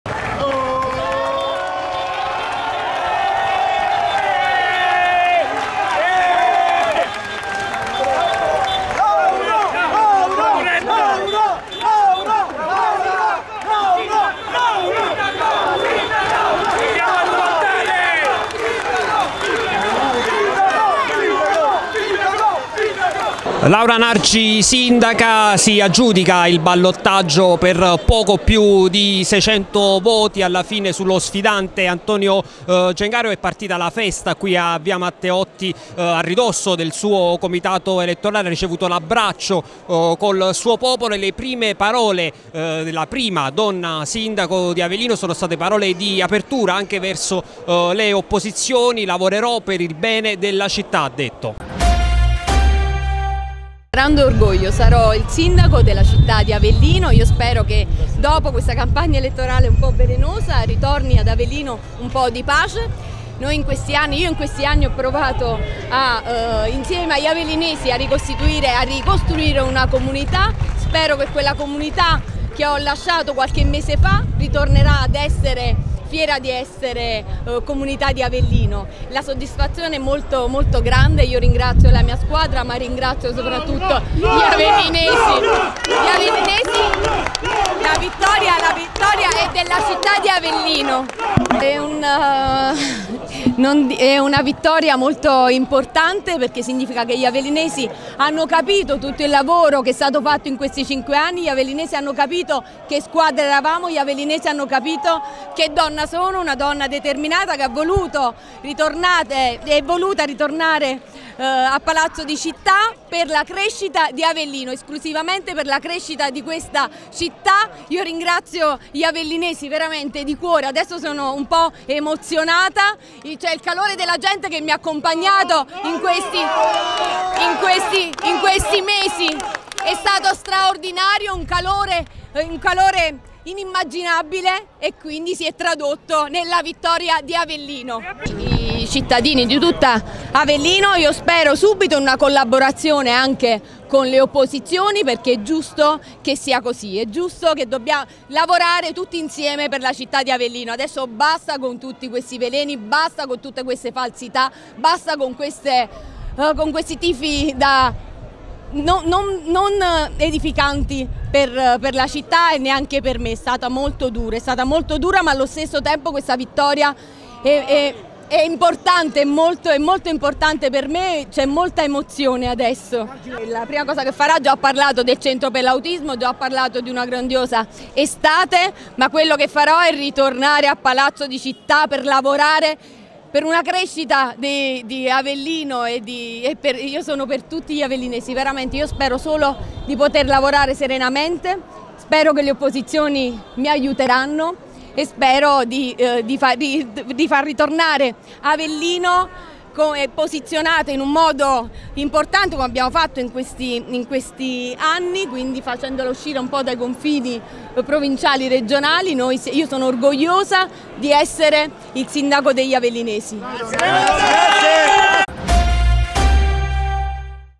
“E’ la vittoria di Avellino. Ora basta veleni, le opposizioni collaborino per il bene della città”
“E’ per me un grande orgoglio essere la prima donna sindaco di Avellino, spero che la città si ricompatti dopo una campagna elettorale al veleno e che torni un po’ di pace nel capoluogo”. Sono passate da poco le 17:00 quando Laura Nargi arriva tra abbracci, cori e applausi a via Matteotti, nei pressi del proprio comitato elettorale, per festeggiare con la sua gente l’elezione a sindaco. Uno scrutinio rapido ha consegnato la vittoria all’ex Vicesindaca di Gianluca Festa, che ottiene la fascia tricolore superando di quasi 900 preferenze lo sfidante Antonio Gengaro.